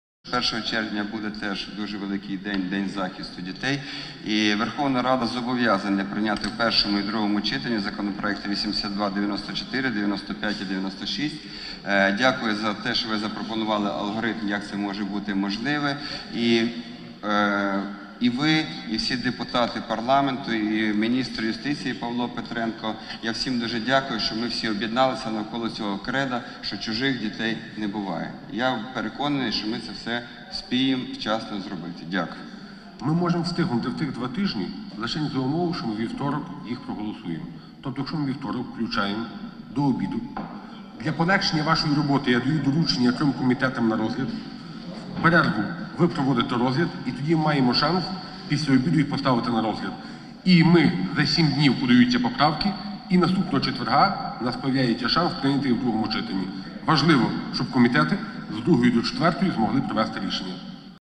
Законопроекти про відповідальність неплатників аліментів мають бути прийняті до Дня захисту дітей
До 1 червня, Дня захисту дітей, Верховна Рада зобов’язана прийняти у першому і другому читанні законопроекти №№8294, 8295 і 8296, які захищають права дітей та посилюють відповідальність неплатників аліментів.

Про це на Погоджувальній раді заявив голова Комітету з питань економічної політики, представник «Народного фронту» Андрій Іванчук.

«Рада зобов’язана прийняти ці законодавчі ініціативи до Дня захисту дітей. Це стане гарним подарунком для сотень тисяч найменших українців», - сказав він.

«Дякую міністру юстиції Павлу Петренку, депутатам, які об’єдналися довкола кредо «Чужих дітей не буває». Я переконаний, що ми встигнемо прийняти ці важливі законопроекти до 1 червня», - наголосив Іванчук.

Спікер парламенту Андрій Парубій, коментуючи виступ Андрія Іванчука, зазначив, що Рада зможе встигнути розглянути ці проекти протягом двох тижнів за умов, що цього вівторка за них проголосують у першому читанні, а наступного четверга, після подання поправок, буде можливість розглянути їх у другому читанні.

Приєднуйся!